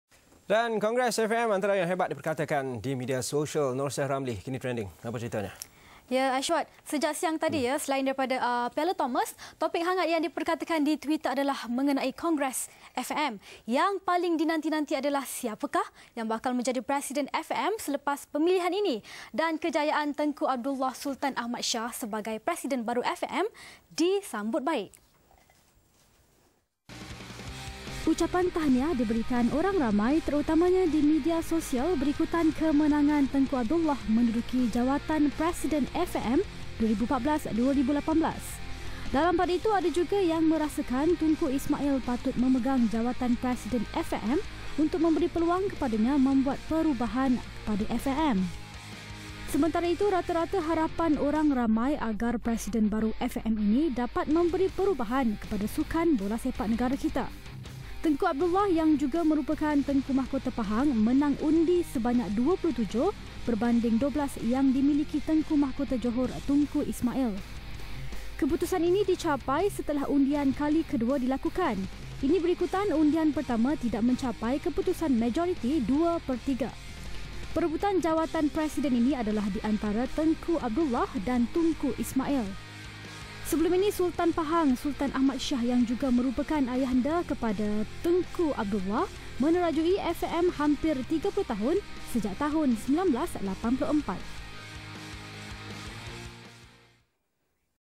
Trending Tengku Abdullah Presiden baru FAM
Sejak siang tadi, topik hangat yang diperkatakan di Twitter adalah mengenai kongres FAM. Yang paling dinanti-nanti adalah siapakah yang bakal menjadi Presiden FAM selepas pemilihan ini. Dan kejayaan Tengku Abdullah Sultan Ahmad Shah sebagai presiden baru FAM disambut baik.